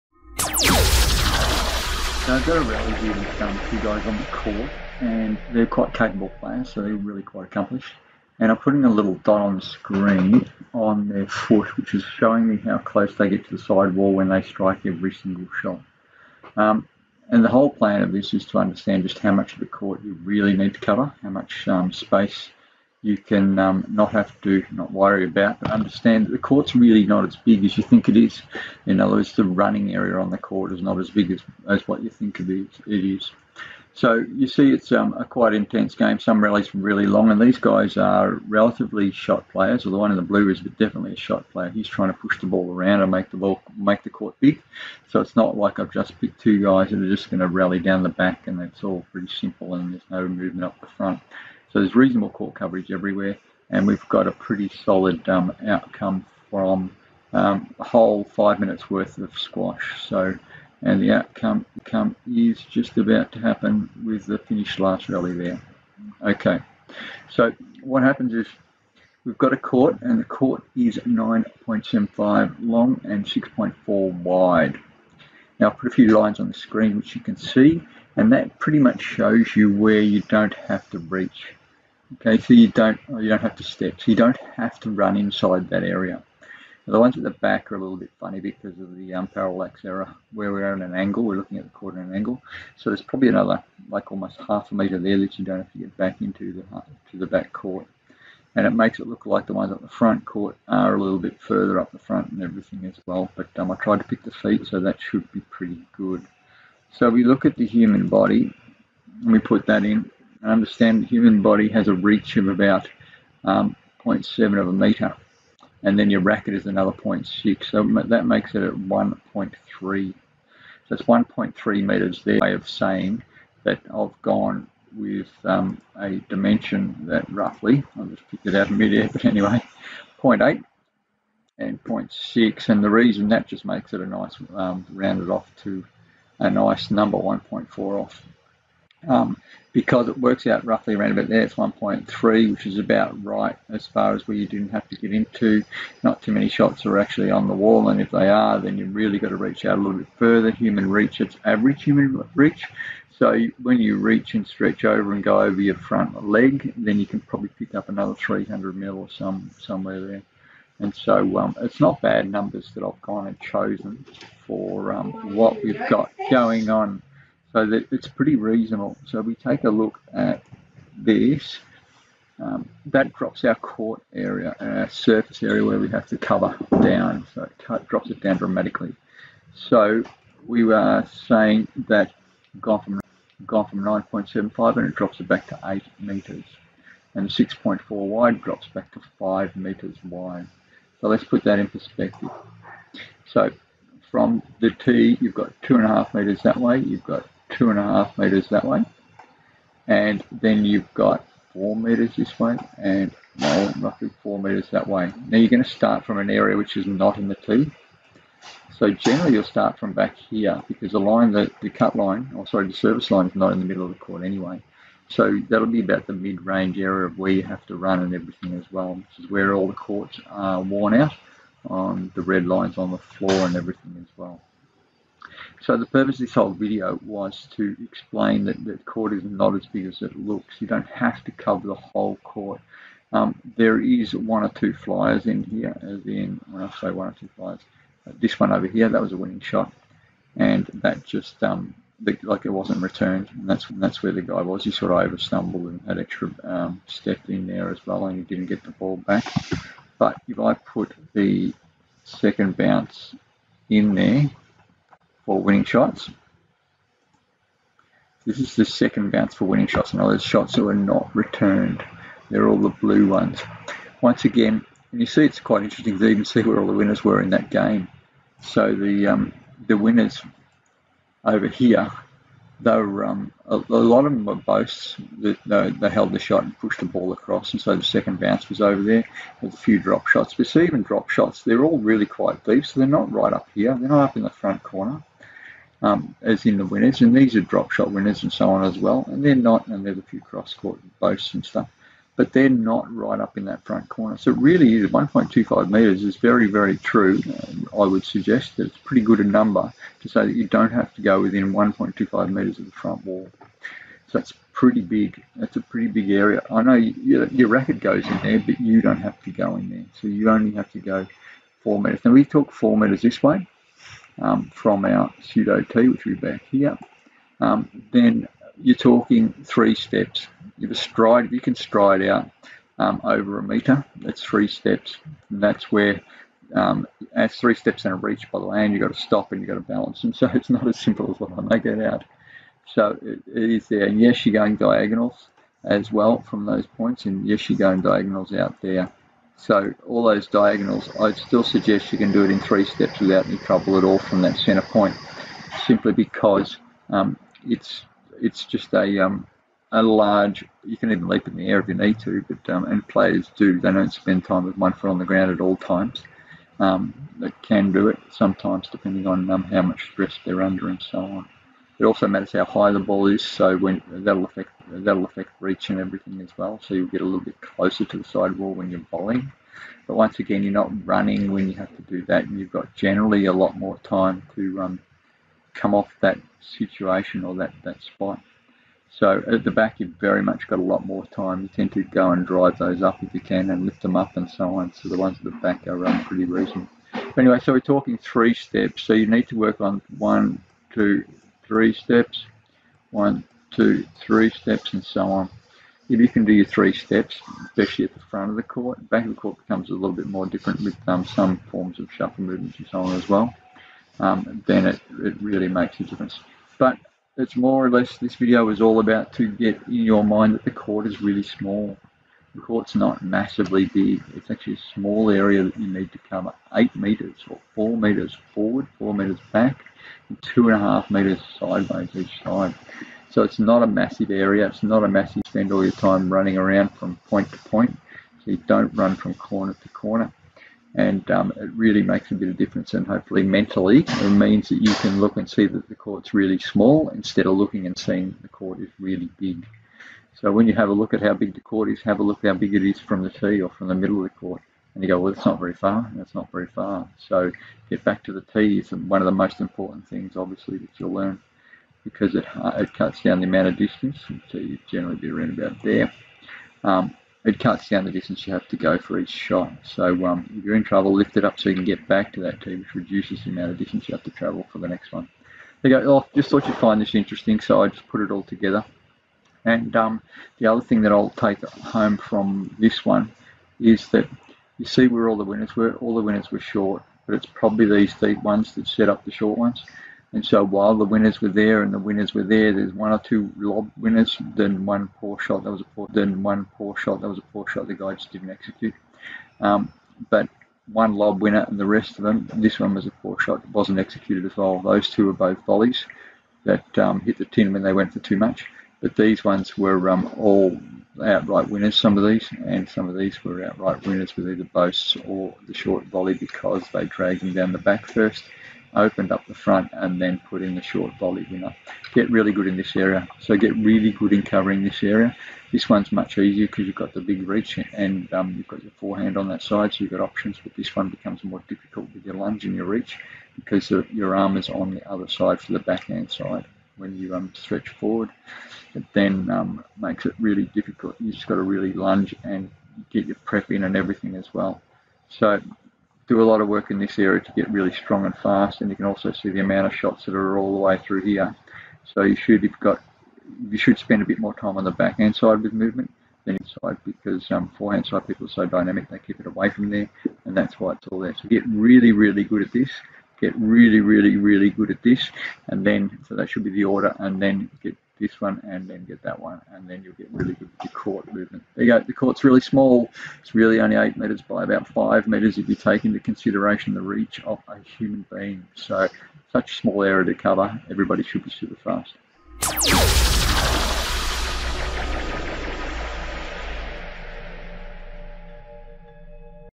Squash is a small court to cover
With reach of your arm and the racquet you do not have to move to the side wall to hit and drop shots come back toward the center of the court by more than one meter.
This makes the running area of the court much smaller so change your thinking that the court is a big area to cover when in reality it is a much smaller area.
This video is about mindset for training and playing, you believe whatever you tell your self, so make it easier for your perception of effort required to get around the court.